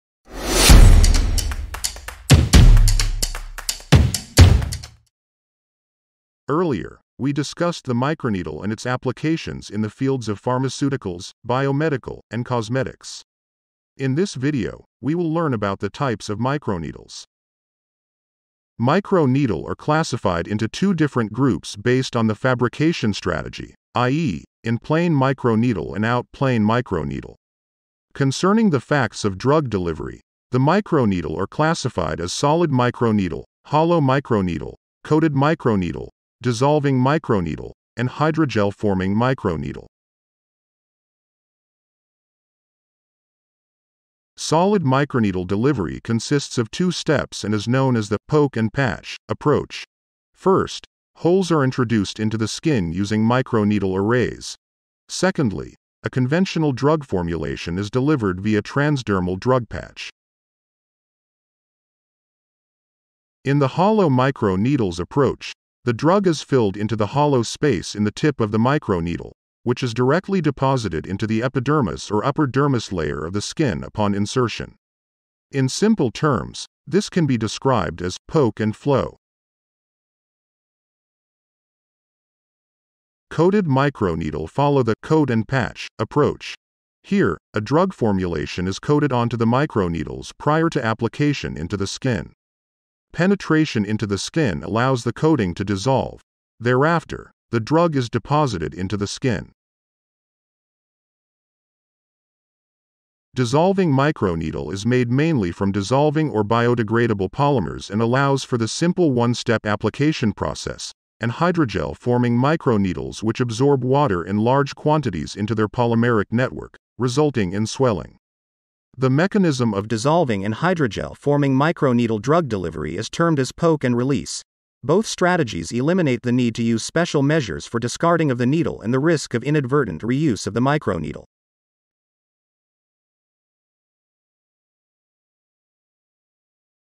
Types of Microneedle || Novel delivery system
Types of Microneedle | Ultimate Guide | Novel  delivery system

Microneedling has become an increasingly popular skincare treatment in recent years, with numerous benefits such as improved skin texture, reduced acne scars, and fine line reduction. However, there are different types of microneedles available in the market, and it can be overwhelming to choose the right one for your skin concern.

Whether you're a seasoned microneedling enthusiast or a first-time user, this guide will equip you with the knowledge to make an informed decision on the best microneedling tool for your skin type and concern. So, grab a cup of tea and tune in to this comprehensive guide on microneedling.Novel delivery system